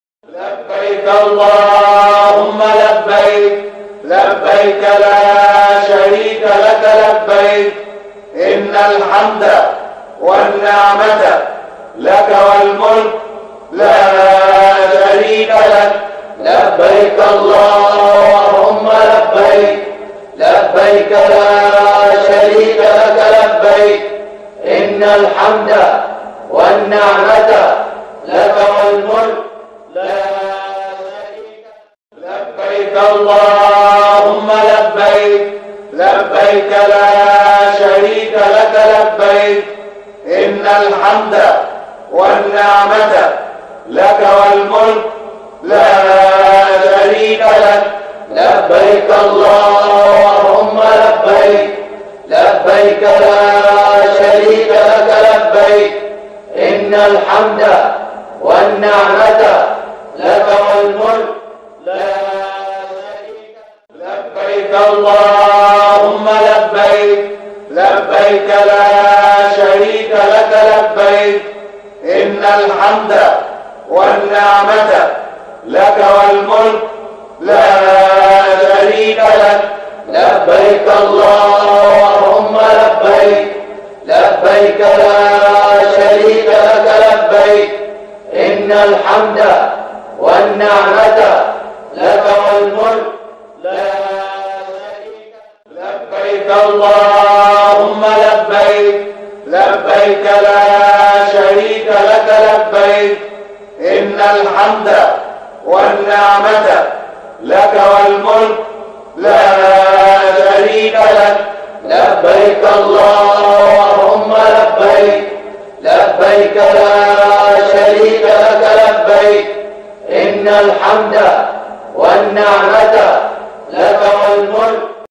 بالصور٠٠ضيوف الرحمن على جبل عرفات لأداء الركن الاعظم فى الحج🕋
بالصور ضيوف الرحمن على جبل عرفات لأداء الركن الأعظم من الحج

القناة الرسمية لكنوز المعرفة

salwa seyam